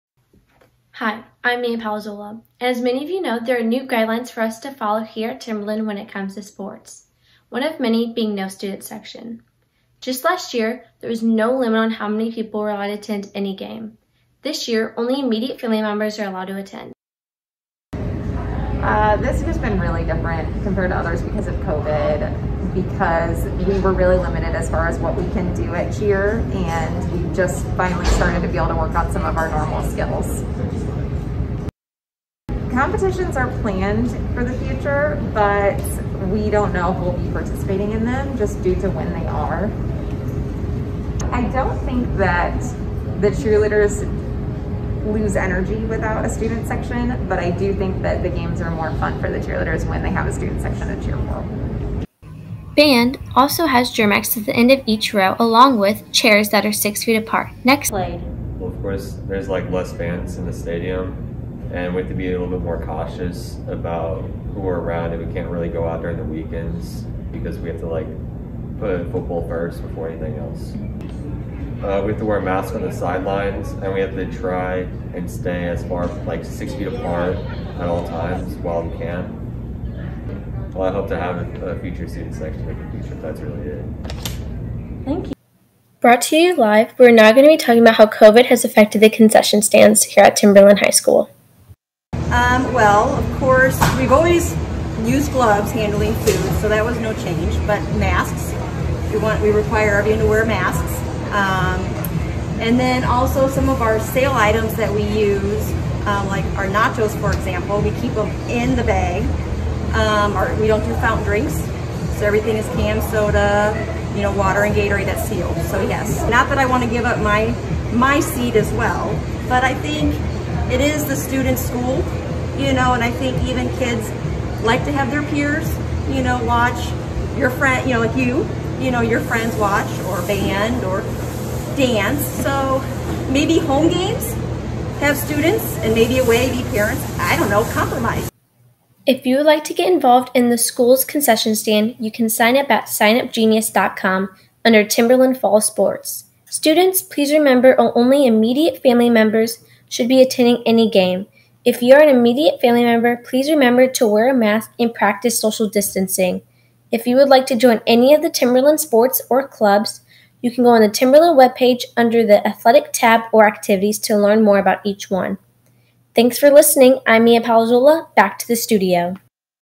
Sports Without A Student Section at Timberland
In this WHTV news story, a broadcast journalism student tells about the 2020 fall season restrictions due to COVID 19.